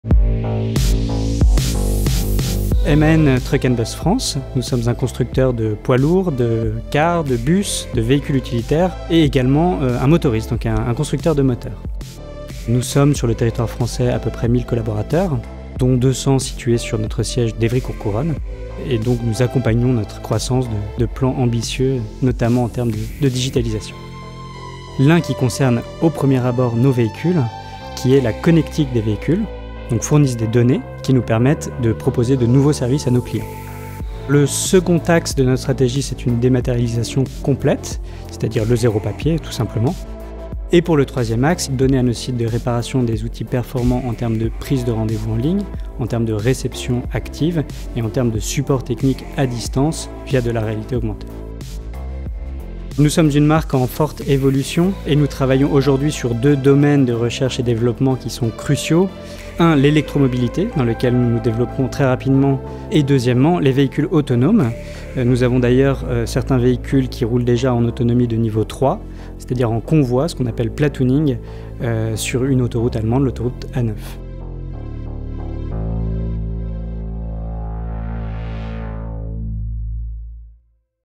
Man Truck & Bus France - Prix Digital - 91 d'Or 2019
MAN propose une large gamme de produits et services. Les prestations s’orientent vers les besoins de ses clients. L'objectif est d’accroître la profitabilité des sociétés de transports par le biais de faibles coûts d’exploitation et d’un service après-vente efficace.

Découvrez l'entreprise récompensée pour le prix Digital pendant la 27ème Cérémonie des 91 d'Or remis par la Communauté d’Agglomération de Grand Paris Sud.